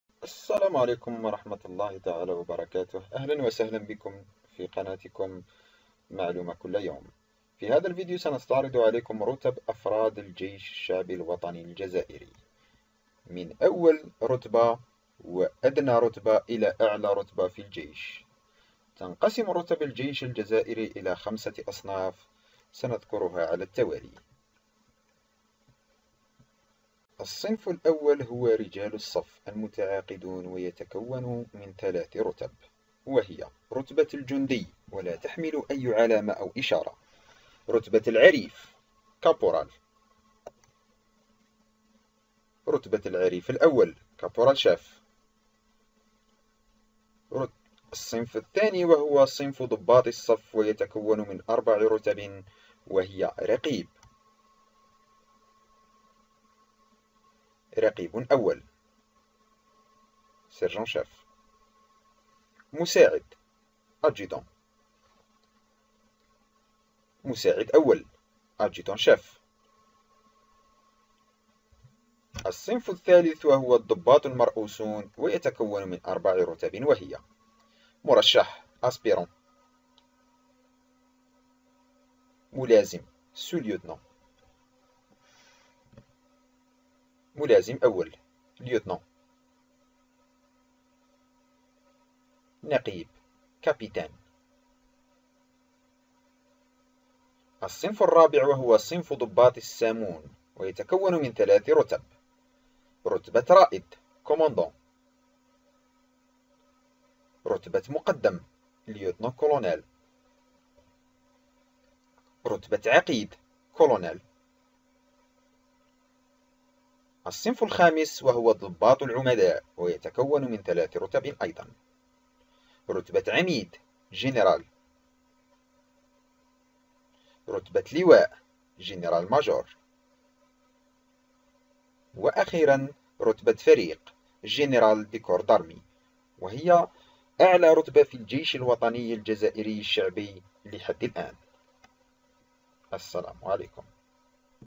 The Algerian People's National Army -- هذه هي رتب أفراد الجيش الوطني الشعبي الجزائري
رتب أفراد الجيش الوطني الشعبي الجزائري بالترتيب
Arrange the members of the Algerian People's National Army in order